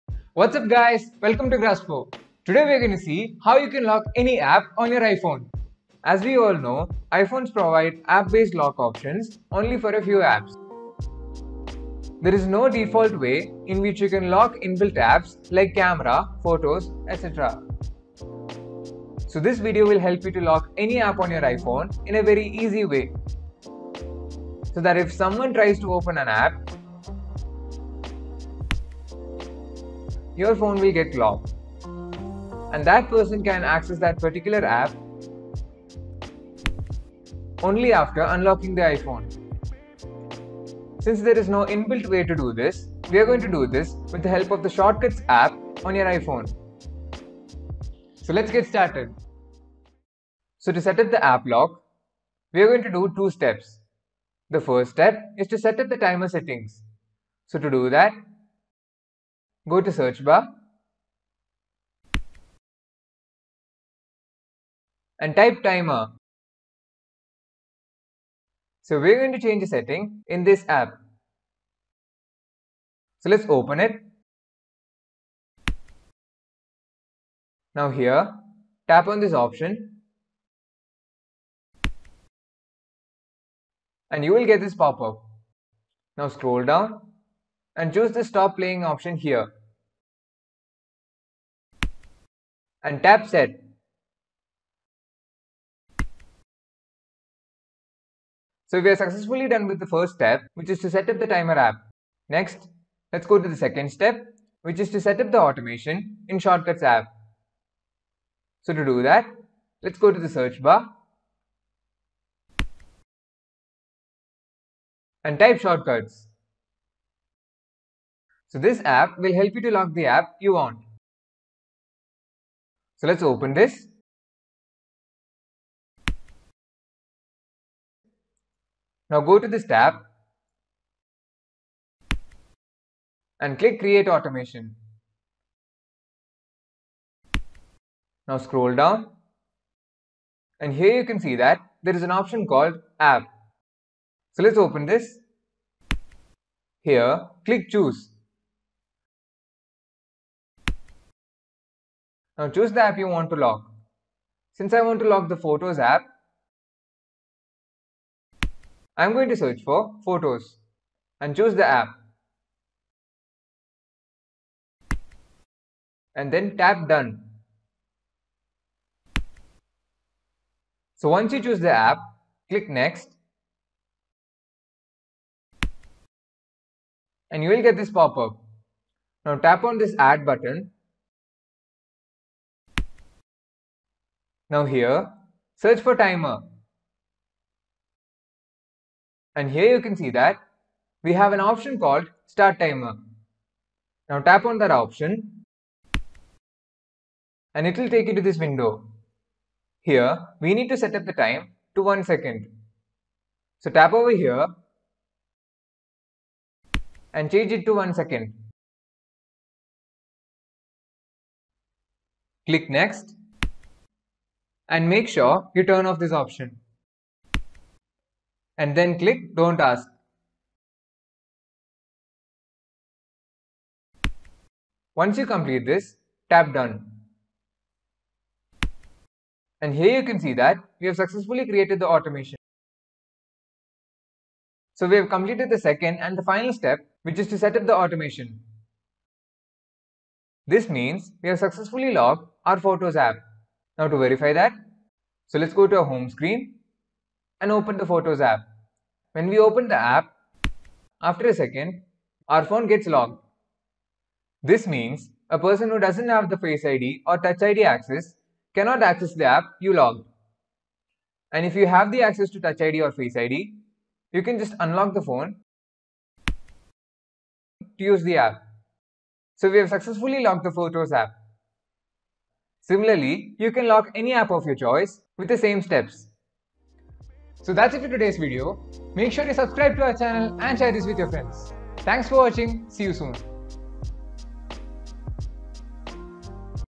How to Lock Apps on iPhone | Lock Apps on iPhone with Face ID or Touch ID
This video shows detailed way on how to Lock Apps on iPhone !
Are you looking to way to Lock App on iPhone using Touch ID or Face ID this the easiest way to do it , this super clear tutorial hells you to easily lock any apps on your iPhone for free , since there is no inbuilt way to do this, we are going to lock it using the  'Shortcuts' app on the iPhone.
Make sure watch this video till the end to know how to do it !! 😀

LockAppOniPhone

Video Content:
0:00 - Intro
0:56- Step 1: Setup the Timer Settings
1:45 - Step 2 : Setup the Automation using Shortcuts app
4:06 - Demo of Locked App - Unlccking using Face ID/ Touch ID
4:38 - Outro

This video will help you to lock any app on your iPhone in a very easy way.

So that, if someone tries to open that app, your phone will get locked,

and that person can access that particular app, only after unlocking the phone.

Since there is no inbuilt way to do this,

We are going to do this with the help of the shortcuts app on your iPhone.

So to setup the app lock,

We are going to do 2 steps

The first step is to setup the timer settings

So to do that

Go to search bar

And type timer

So we are going to change a setting in this app

So let's open it

Now here,

Tap on this option

And You will get this popup

Now scroll down

And choose the “stop playing” option here.

And tap set.

So we are successfully done with the first step, which is to set up the timer app.

Next let's go to the second step

which is to set up the automation, in shortcuts app.

Lets go to the search bar

And type shortcuts

So this app will help you to lock the app you want

So let's open this

Now go to this tab

And click click ‘create automation’

Now Scroll down

And here you can see that there is an option called ‘app’

Here click choose

Now choose the app you want to lock

Since i want to lock the ‘photos’ app

I am going to search for ‘photos’

And choose the app

And then tap done.

So once you choose the app.

Click next

Now tap on this add button

Now here, search for ‘timer’

And here you can see that

we have an option called ‘start timer’

Now tap on that option

And It will take you to this window

Here we need to setup the time to one second

So tap over here and change it to one second

Now turn off this option

and tap don't ask

Once you complete this,

tap done

And here you can see that we have successfully created the automation

So we have completed the second and the final step which is to set up the automation

This means we have successfully locked our photos app.

Now to verify that

Let's open the photos app

So let's go to our home screen and open the photos app

Here you can see that, when we open the app, after a second, our phone gets locked

This means, a person who doesn’t have the face id or touch id access

cannot access the app you locked.

And If you have the access to face id or touch id,

 you can just unlock the phone to use the app.

So we have successfully locked the photos app.

Similarly you can lock any app of your choice with the same steps.

So that's it for todays video

If you want to know how to set any song as ringtone you can watch this video